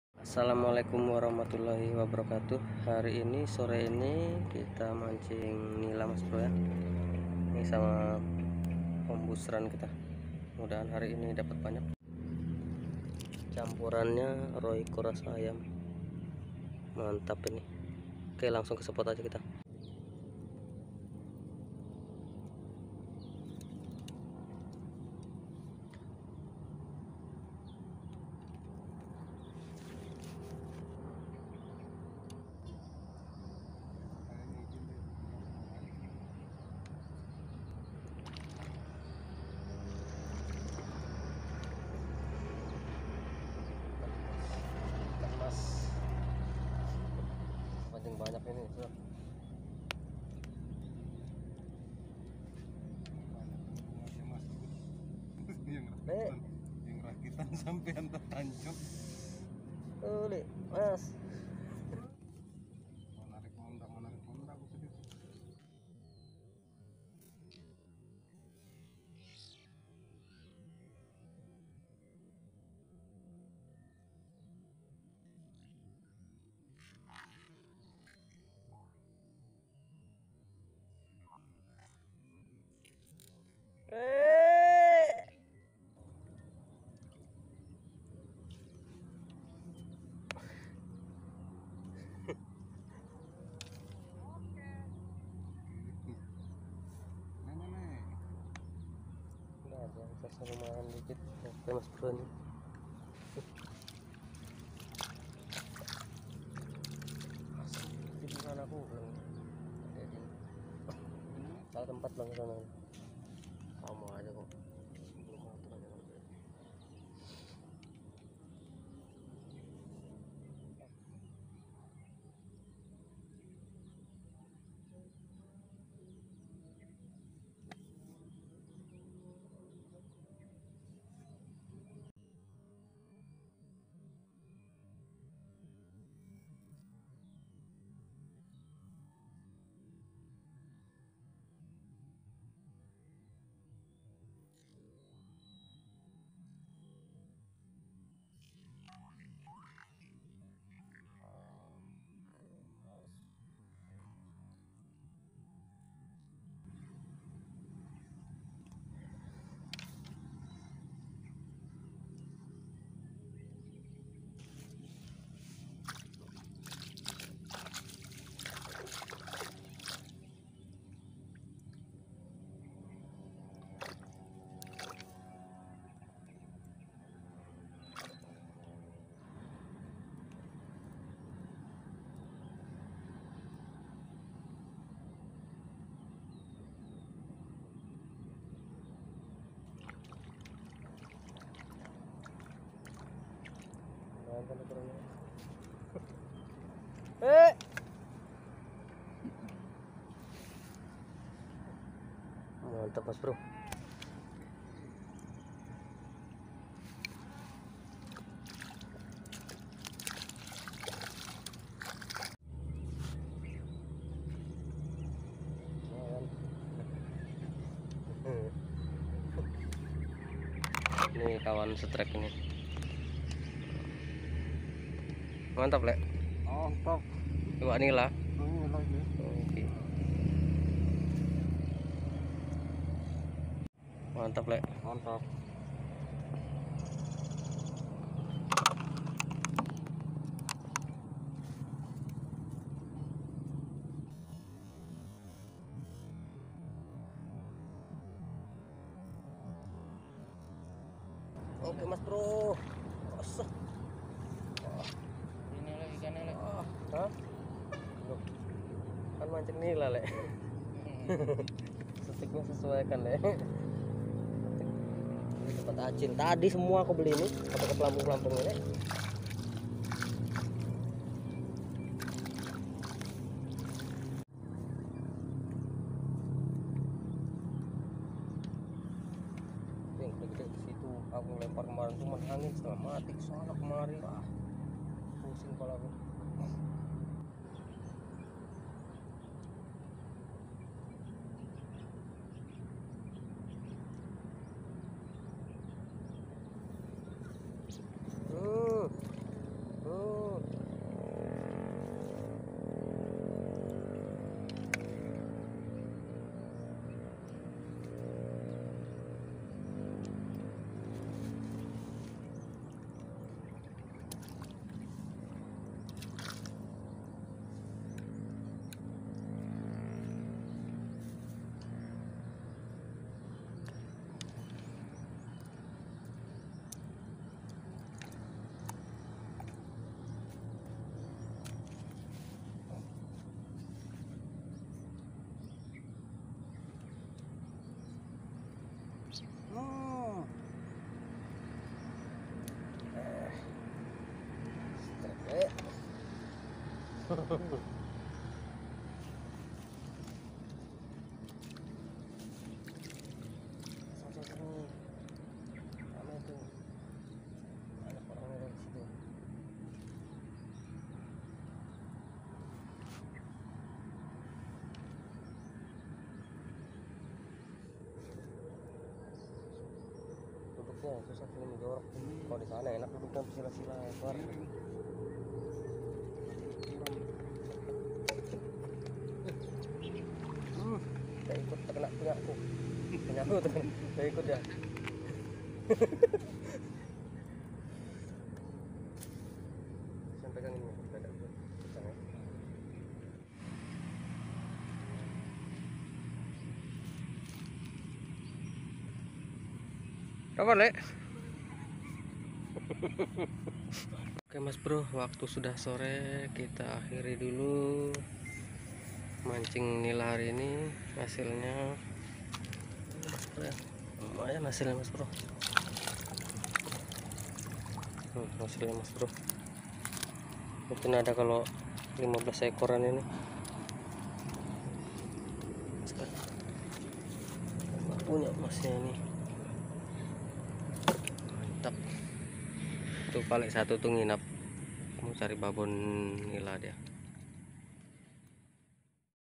JORANKU NYARIS HILANG || UMPAN JITU BUAT MANCING NILA!!!
MAMCING NILA PAKAI UMPAN LUMUT DICAMPUR ROICO...